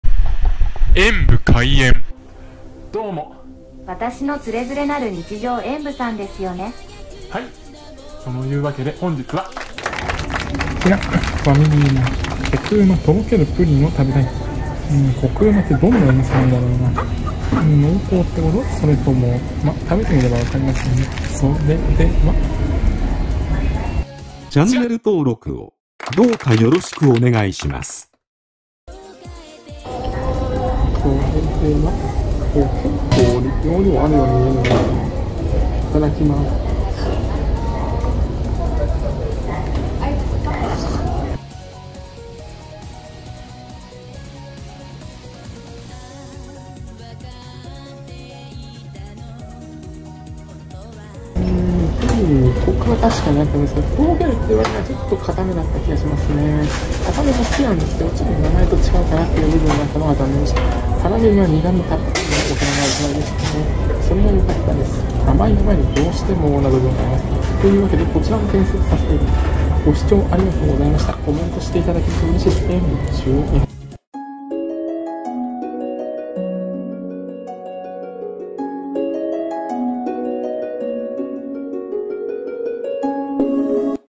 ファミリーマート コク旨とろけるプリンを食べてみた。つまりは濃厚って事でOKI？
ファミリーマート、コク旨とろけるプリンを食べてみました。コクが有るって事はつまり、濃厚という認識でよろしいのでしょうか？いや違うか？